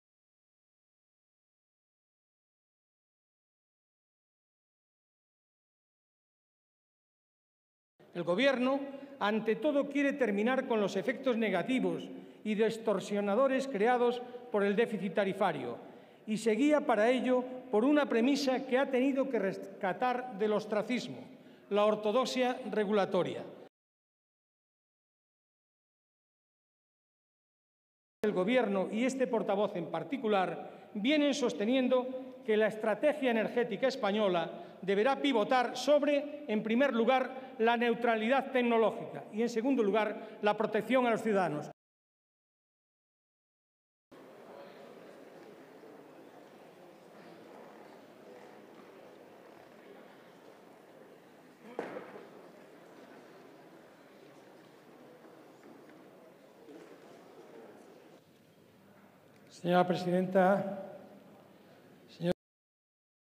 "Los principios de la reforma son la neutralidad tecnológica y la protección a los ciudadanos"
El portavoz de Energía del Grupo Parlamentario Popular, Antonio Erías, señaló hoy que el "objetivo prioritario es acabar con el déficit anual de tarifa y evitar que vuelva a aparecer" a través del camino de la "ortodoxia regulatoria".